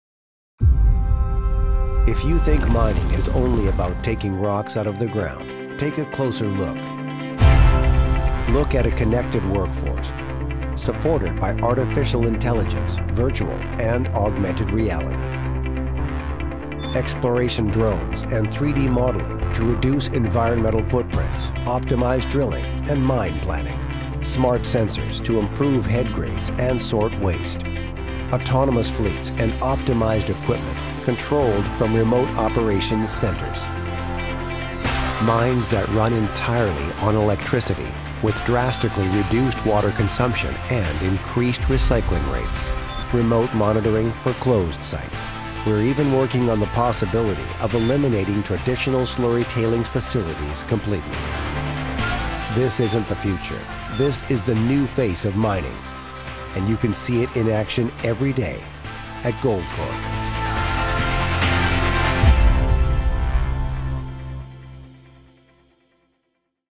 Take a Closer Look - Innovation at Goldcorp
Highlight of Goldcorp's innovation initiatives. We innovate at every stage of the mine lifecycle to make our operations more efficient, reduce our impact on the environment and identify exploration targets more effectively. To learn more how innovation supports our strategic initiatives,